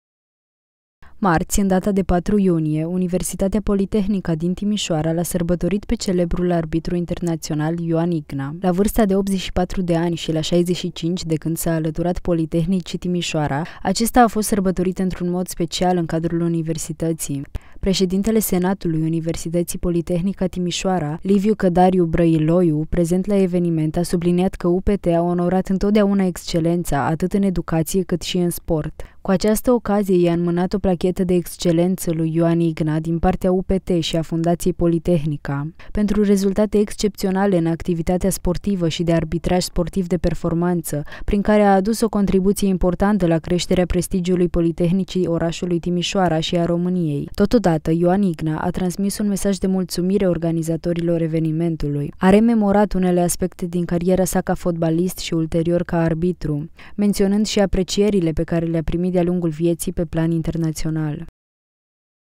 Celebrul arbitru Ioan Igna, sărbătorit la Timișoara
Marți, în data de 4 iunie, Universitatea Politehnica din Timișoara, l-a sărbătorit pe celebrul arbitru internațional, Ioan Igna. La vârsta de 84 de ani, și la 65 de când s-a alăturat Politehnicii Timișoara, acesta a fost sărbătorit într-un mod special în cadrul Universității. Președintele Senatului Universității Politehnica Timișoara, Liviu Cădariu-Brăiloiu, prezent la eveniment, a subliniat că UPT a onorat întotdeauna excelența, atât în educație, cât și în sport. Cu această ocazie i-a înmânat o Plachetă de Excelență lui Ioan Igna, din partea UPT și a Fundației Politehnica, ,,Pentru rezultate excepționale în activitatea sportivă și de arbitraj sportiv de performanță, prin care a adus o contripuție importantă la creșterea prestigiului Politehnicii, orașului Timișoara și a României.”